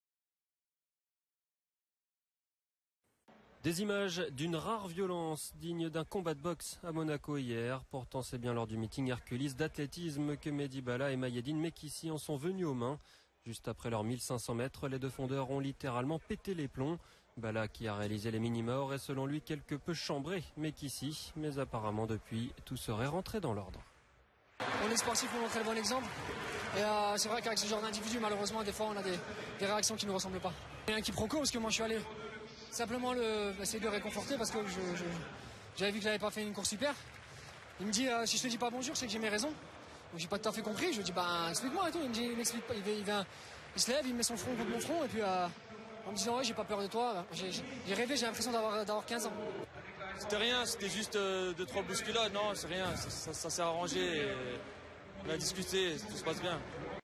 Bagarre entre Baala et Mekhissi lors du meeting Herculis
"combat de boxe" entre Mahdi Baala et Mahiedine Mekhissi à l'arrivée du 1500 mètres du meeting Herculis de Monaco